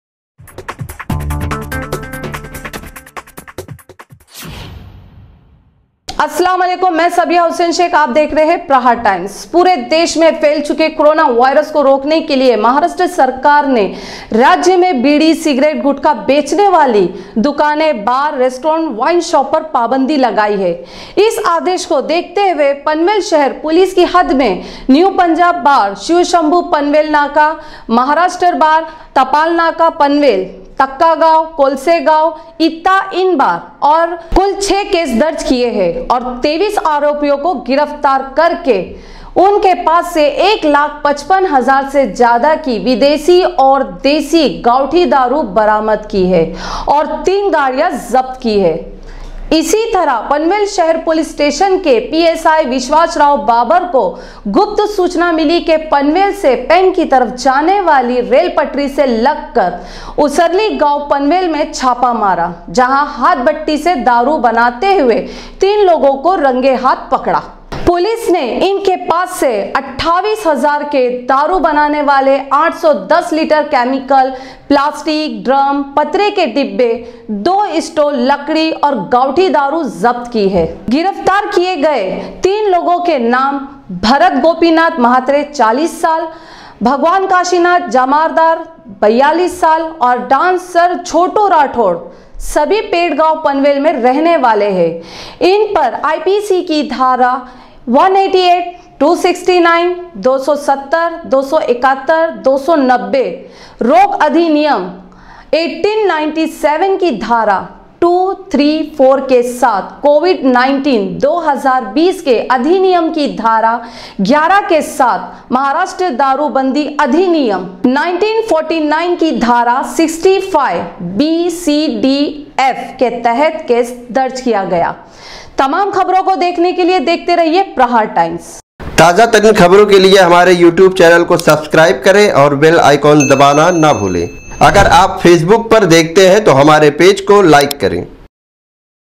हाथ भट्टी की दारू बनाते तीन को पकड़ारेल की पटरियों के किनारे बना रहे थे दारू||Aapka Prahar Times
हाथ भट्टी की दारू बनाते तीन को पकड़ा
रेल की पटरियों के किनारे बना रहे थे दारू